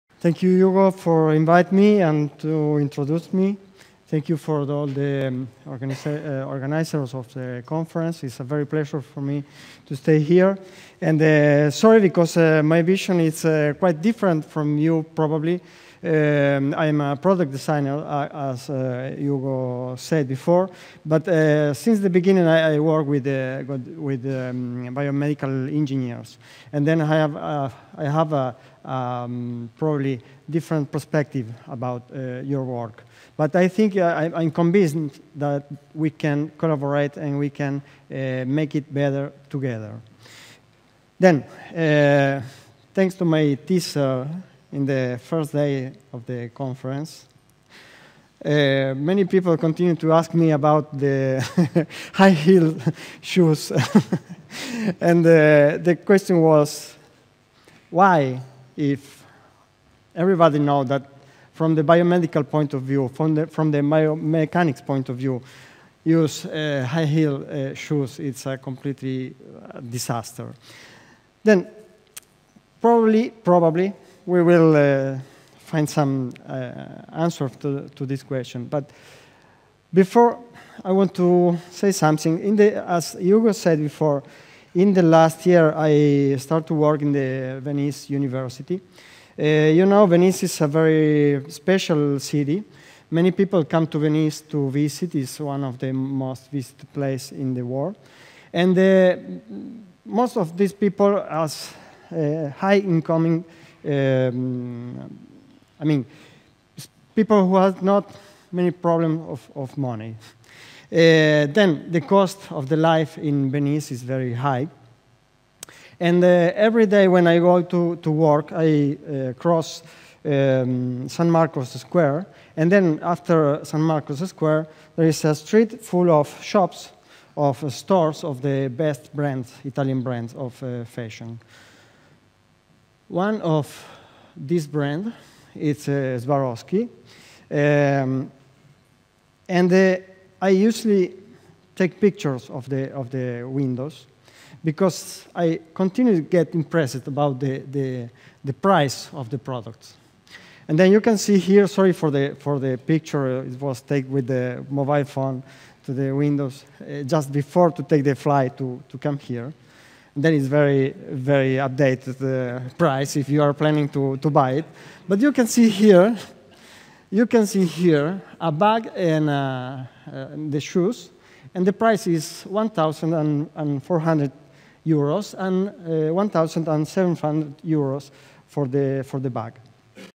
"The New Paradigm of Industrial Design for Disability" Prof. Maximiliano Romero (BIOSTEC 2018)
Title:  The New Paradigm of Industrial Design for Disability

Keynote Lecturer: Maximiliano Romero
Presented on: 21/01/2018, Funchal, Madeira, Portugal

Abstract: Industrial design was born with the industrial revolution thanks to the intention of consciously design objects for everyday use. Since its inception, industrial design has had to mediate between the needs of the users and the production capacities of the industry. Before that revolution, objects were crafted manually by the end-user themselves, as a response to their specific needs, or by artisans with special manual skills who knew of those needs. Mass production has allowed to reduce costs for the industry, but at the same time forced people to use generic products that are not tailor-made. Typically, it is the man who has to adapt to the product and not vice versa. Only in recent years have the user needs become important enough to overcome technical/economical restrictions. Today, the field of Product Design is highly evolved, and several methodologies have been developed to guide the complex process of creating new products. Much of them consider the user requirements superficially and are guided mainly by technological innovation. In market paradigm of mass production, a new product is often produced in hundreds of thousands. When a design mistake occurs, it too is reproduced by hundreds of thousands. If a new product is a commercial flop, the result is tons of trash. In order to reduce this risk, data about user requirements are collected by well-known User Research Methodologies, which tend to increase the likelihood that a given product will be accepted by the public, and thus commercially successful. In the field of disability, markets are much smaller than “consumer” products. Thus, the main impact on the design of a product for disability are from: higher production costs and the need for adaptation. Products for disability, often are morphologically complex because they need adjustments to the specific user needs. Frequently, with the goal of lengthening the life time of the product, stronger materials, robust design and some adjustment capabilities are used. As a result, ugly products are frequently produced, with a consequent stigmatization of the disability status. Needless to say, the worsening of such a situation increases inversely proportional to the user income and strength of the public health system. Today, we should consider a new aspect in relation with this field: Desktop Digital Manufacturing approach. Are we coming back to a pre-industrialization era? How could the Assistive Technology market change in the proximate future?

Presented at the following Event:
BIOSTEC, 11th International Joint Conference on Biomedical Engineering Systems and Technologies